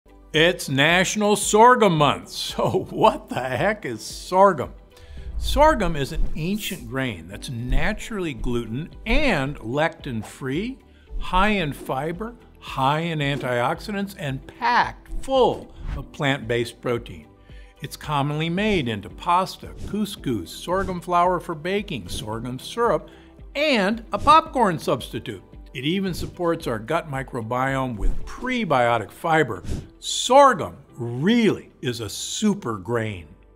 It's Sorghum Month! The Super-Grain explained
It's Sorghum Month! Celebrate with Dr. G and stock up on some sorghum products.
Save 25% on all Sorghum products this month with code JUNESALE25